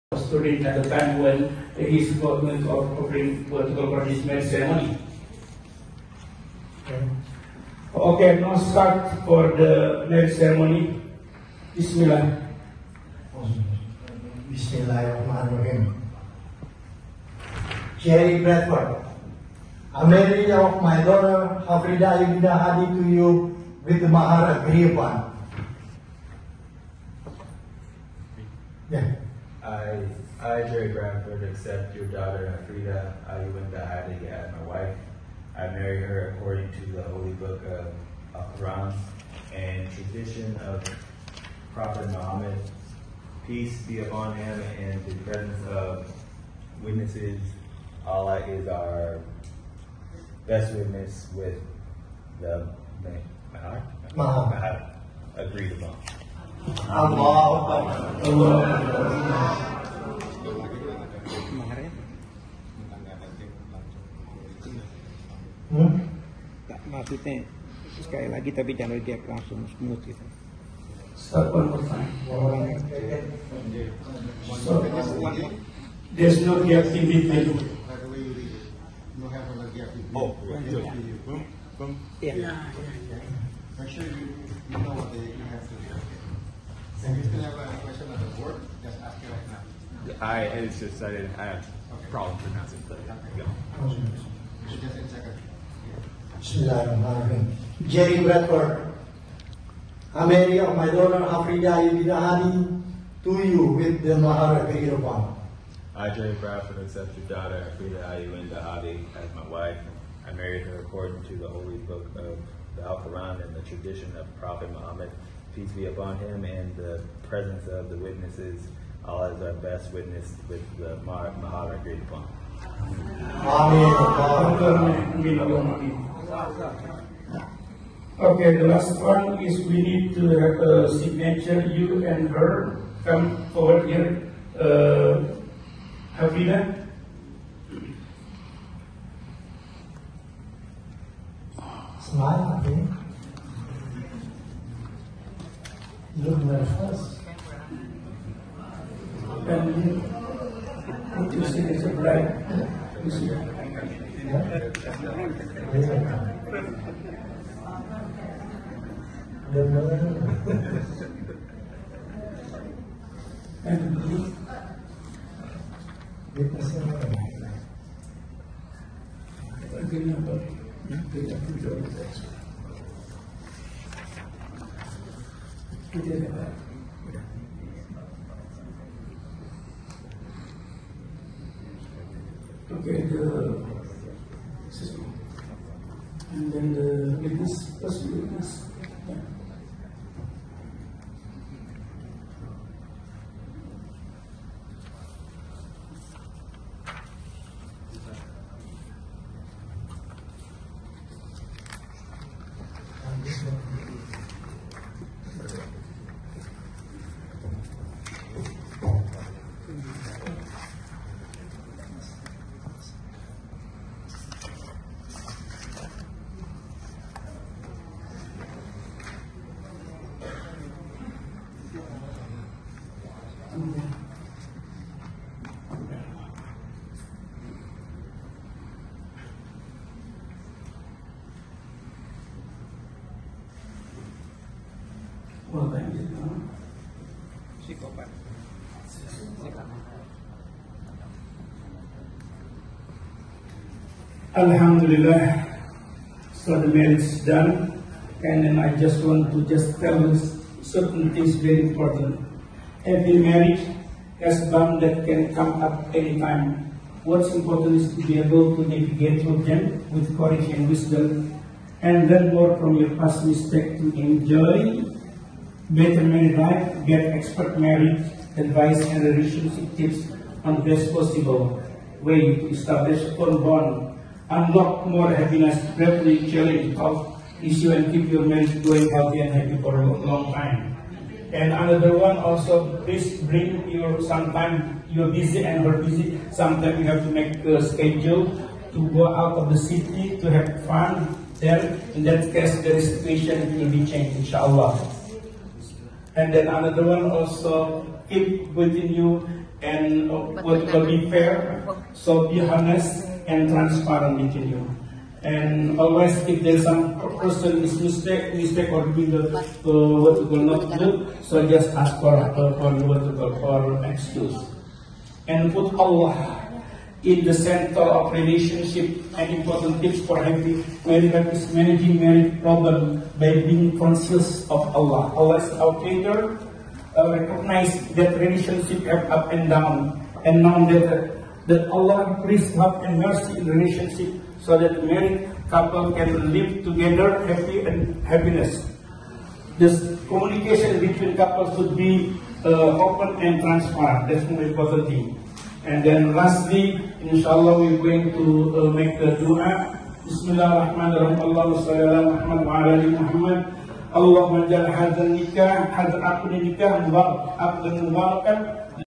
Wedding Day/Pernikahan Hafrida & Jerry on Friday, Oct 21-'22 at Masjid At Thohir LA CA USA. Part 3/6
CONGRATULATIONS! HAPPY WEDDING " BAARAKALLAHU LAKA WABAARAKA 'ALAYKA WA JAMAA' BAYNAKUMAA FII KHAIR

Kalian Tidak Akan Percaya... Apakah ini Amerika!!!